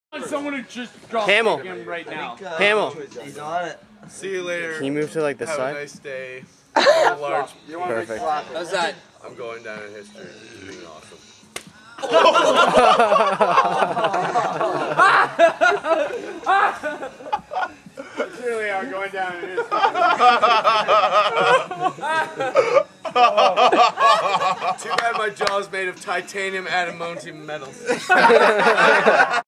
Charles Wins a Slap Bet
Charles wins a slap bet and Nick pays the price